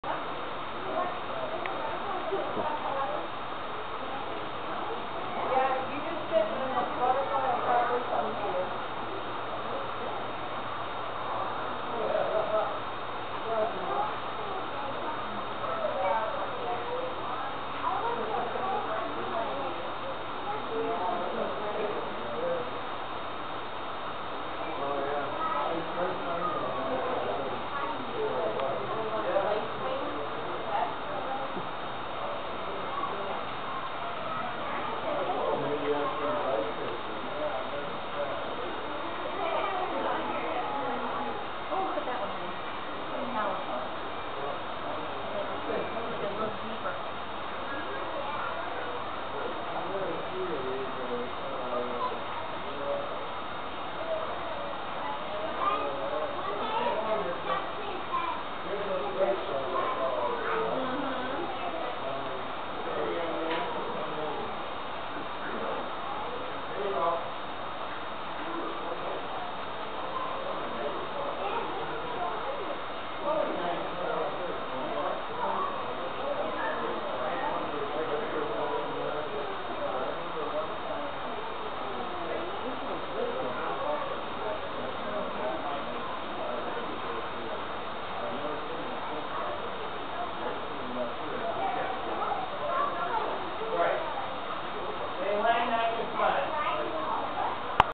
Butterfly House Part3
Carl & Sally having fun, while chasing the butterflies.  Watch out for the big black & white Ricepaper butterfly.

Butterfly House on Mackinac Island.